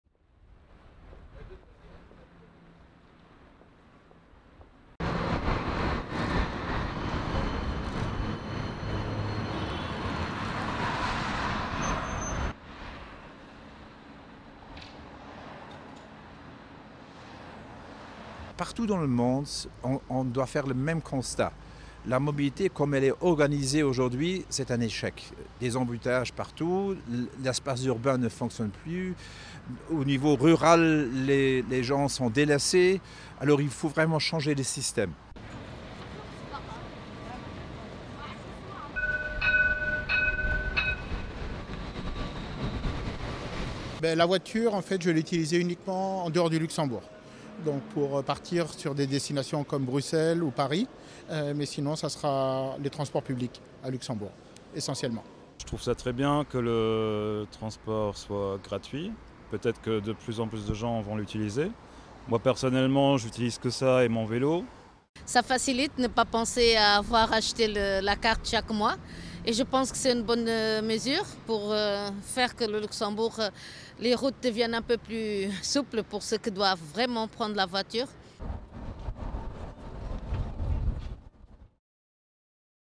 Luxembourg: premier pays à offrir la gratuité des transports publics | AFP News
Le Luxembourg devient le premier pays au monde à offrir la gratuité des transports publics. Ce petit et riche pays de l'UE s'efforce d'aider les travailleurs moins bien lotis et de réduire le trafic routier.